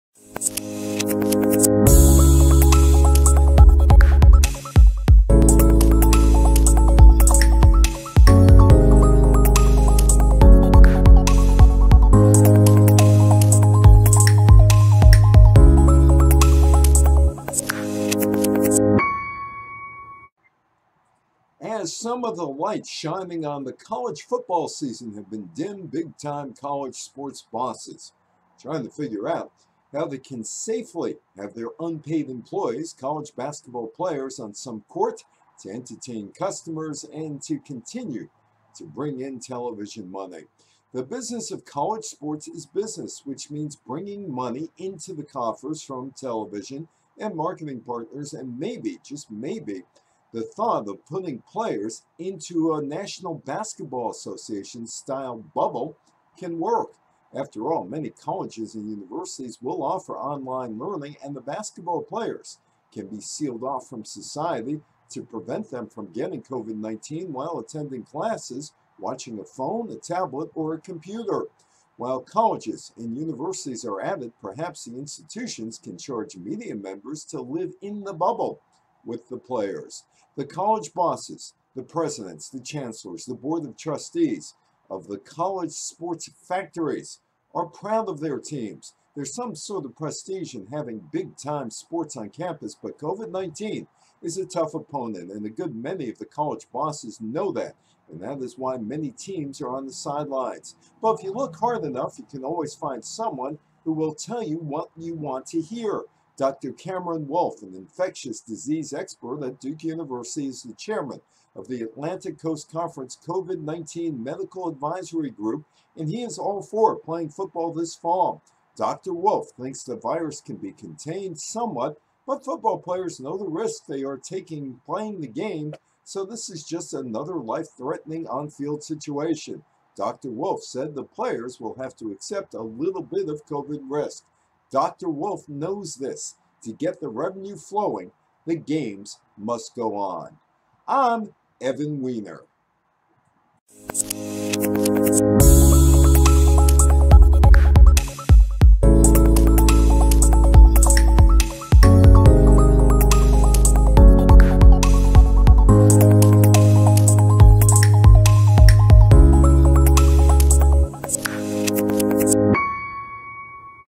Is It Time To Put College Basketball Players Into A Bubble To Save The Season?
COVID-19 is a difficult opponent.